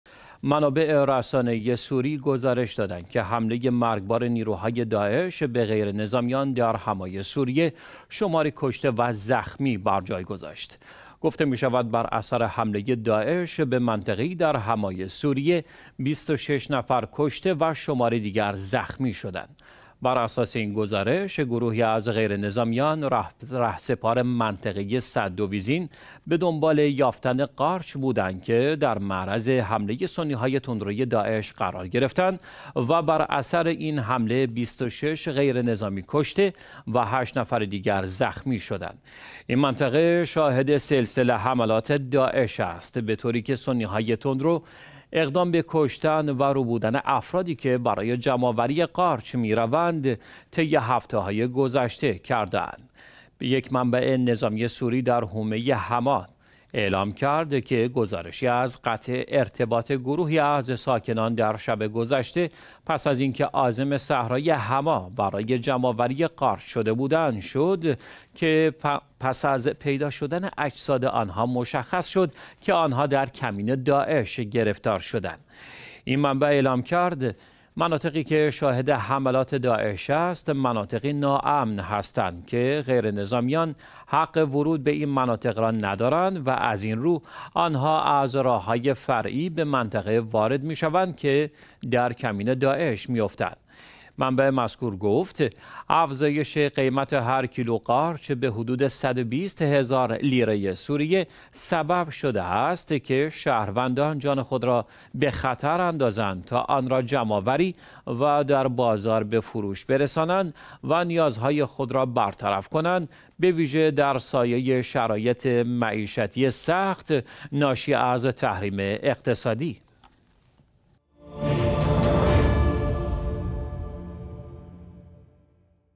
حمله خونین سنی های تندروی د۱عش به حماه سوریه
منابع رسانه ای سوری گزارش دادند که حمله مرگبار نیروهای د۱عش به غیر نظامیان در حماه سوریه شماری کشته و زخمی برجای گذاشت.
گفته می شود بر اثر حمله د۱عش به منطقه ای در حماه سوریه ۲۶ نفر کشته و شماری دیگر زخمی شدند.
بر اساس این گزارش، گروهی از غیر نظامیان رهسپار منطقه سد دویزین به دنبال یافتن قارچ بودند که در معرض حمله سنی های تندروی د۱عش قرار گرفتند و بر اثر این حمله ۲۶ غیرنظامی کشته و هشت نفر دیگر زخمی شدند.
این منطقه شاهد سلسله حملات داعش است به طوری که سنی های تندرو اقدام به کشتن و ربودن افرادی که برای جمع آوری قارچ می روند طی هفته های گذشته کرده اند.
یک منبع نظامی سوری در حومه حماه اعلام کرد که گزارشی از قطع ارتباط گروهی از ساکنان در شب گذشته پس از اینکه عازم صحرای حماه برای جمع آوری قارچ شده بودند، شد که پس از پیدا شدن اجساد آنها مشخص شد که آنها در کمین داعش گرفتار شدند.
این منبع اعلام کرد که مناطقی که شاهد حملات د۱عش است مناطقی نا امن هستند که غیرنظامیان حق ورود به این مناطق را ندارند و از این رو آنها از راههای فرعی به منطقه وارد می شوند و در کمین داعش می افتند.
منبع مذکور گفت: افزایش قیمت هر کیلو قارچ به حدود ۱۲۰ هزار لیره سوریه، سبب شده است که شهروندان جان خود را به خطر اندازند تا آنرا جمع آوری و در بازار به فروش برسانند و نیازهای خود را برطرف کنند به ویژه در سایه شرایط معیشتی سخت ناشی از تحریم اقتصادی.